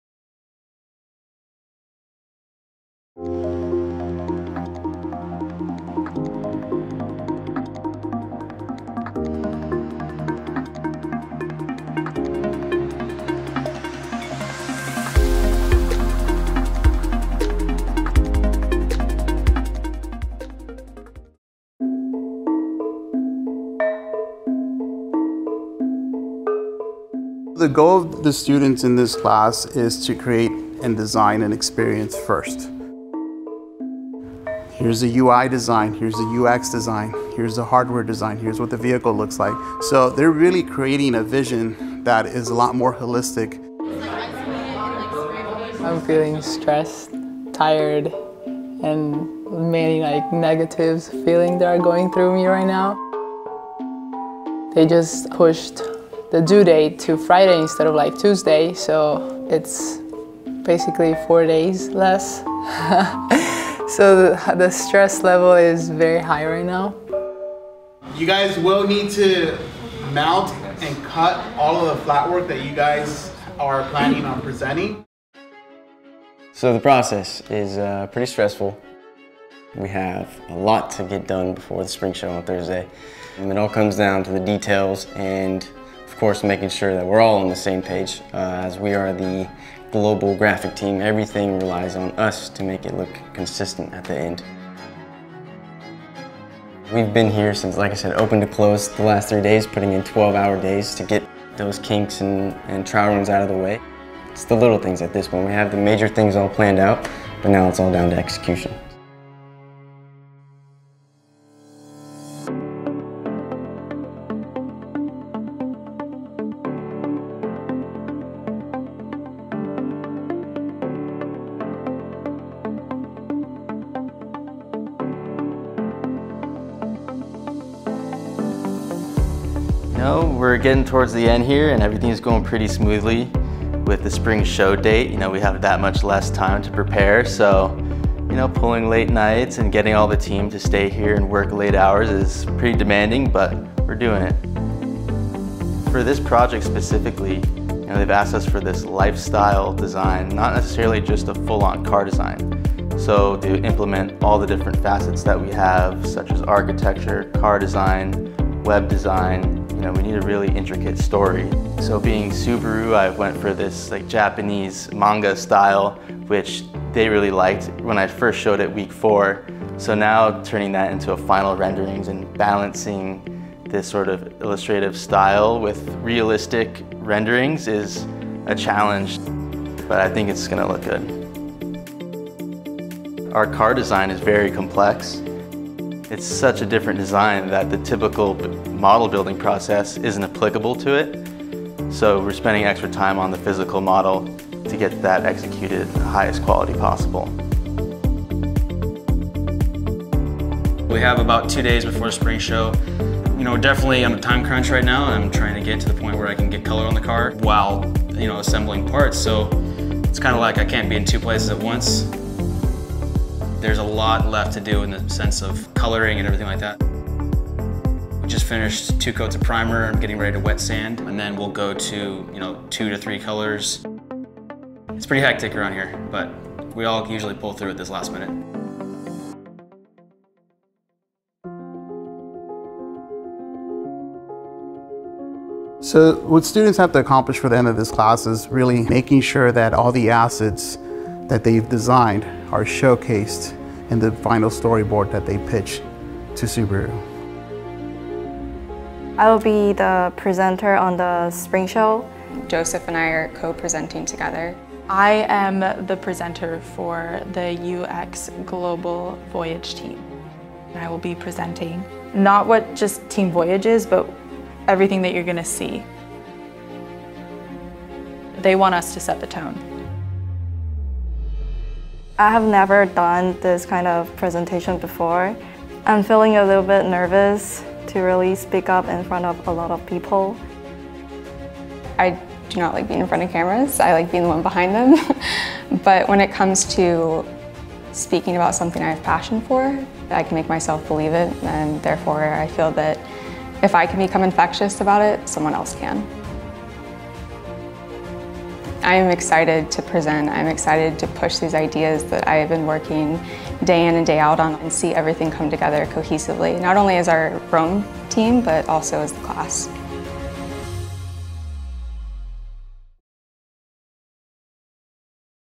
Academy of Art University | The Subaru Project – Time Crunch
Episode 5 of 6. The deadline for final presentations has been pushed up, and our students are feeling the heat. Teams are putting in 12-hour days and pulling late nights, but their holistic vision is coming together. Beyond the car design, the challenge has evolved into a cohesive lifestyle brand that looks to the future. With the major components almost finalized, it all comes down to the details now.